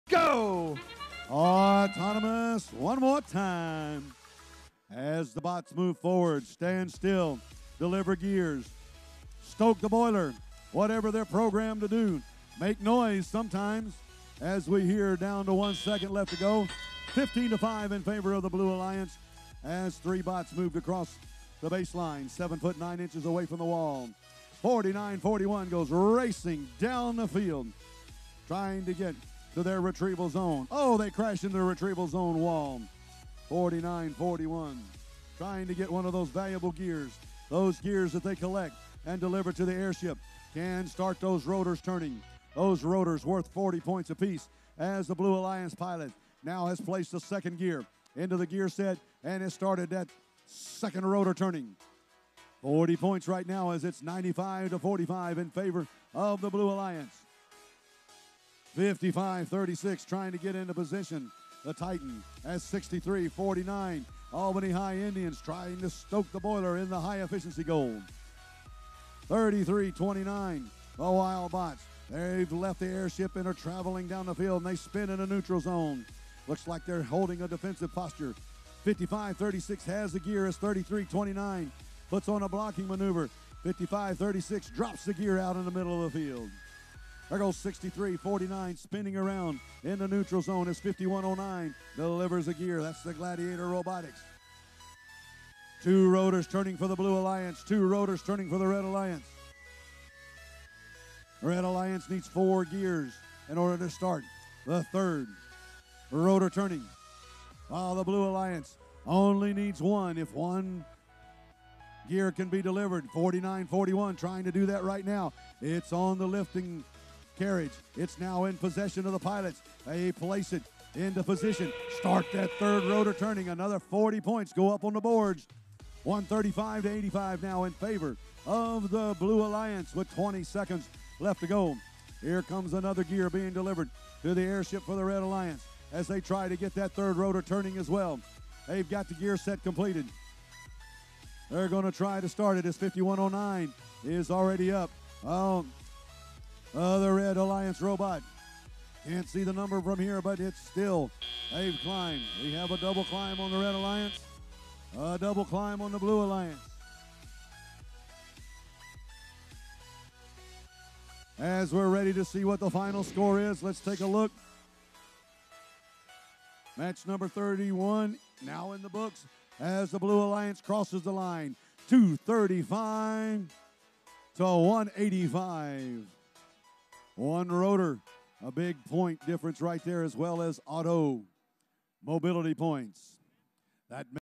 2017 Peachtree District Albany Qualifier - Qualification Match 31
Footage of the 2017 Peachtree District Albany Qualifier is courtesy of the GA FIRST.

Blue Alliances (5632, 4941, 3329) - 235

To view match schedules and results for this event, visit The Blue Alliance Event